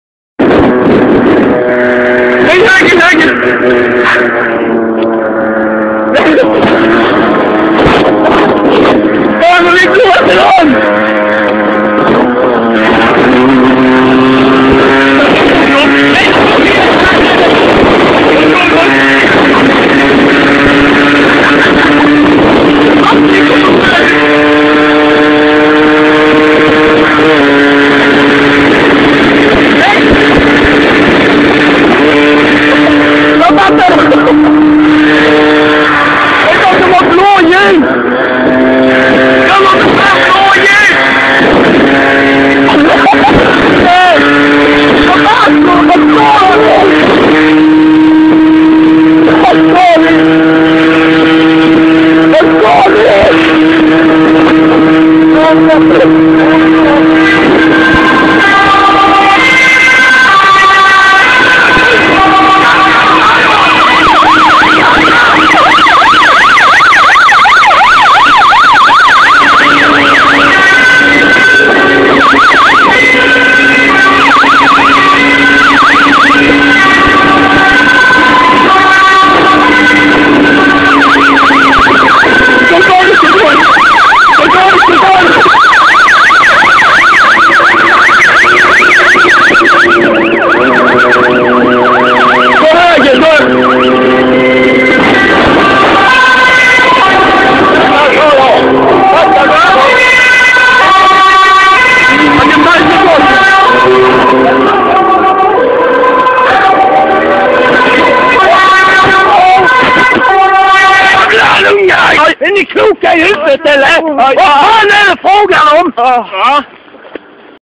Kalle och emil jagade av galna svenska poliser
Kringlan och emil jagade.
Tyvärr så bröt Kringlan handleden under den omdisskuterade jakten!